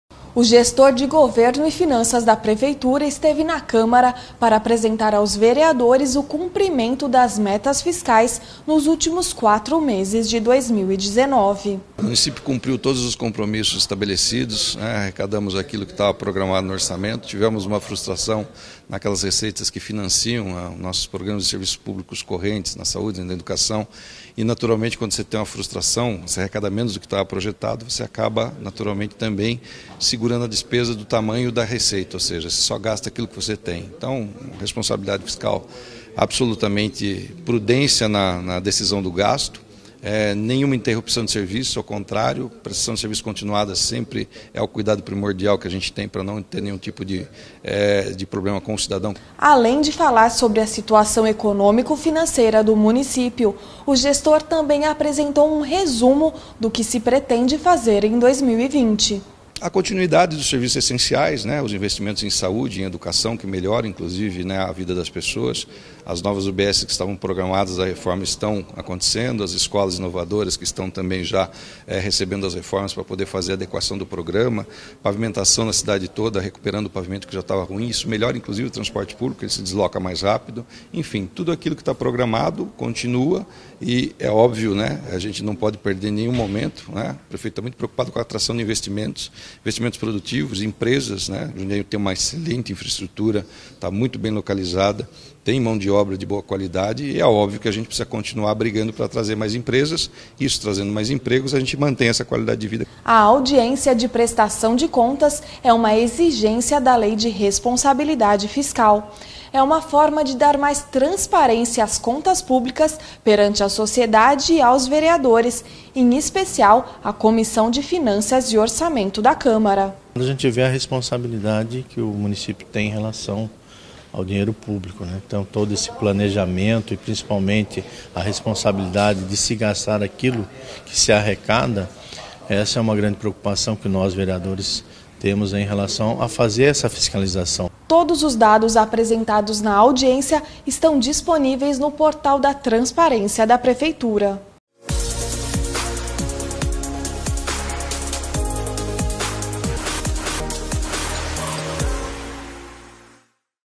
Audiência Pública - Metas Fiscais 3Q -02.03.2020
O Gestor de Governo e Finanças da Prefeitura esteve na Câmara para apresentar aos vereadores o cumprimento das metas fiscais nos últimos quatro meses de 2019.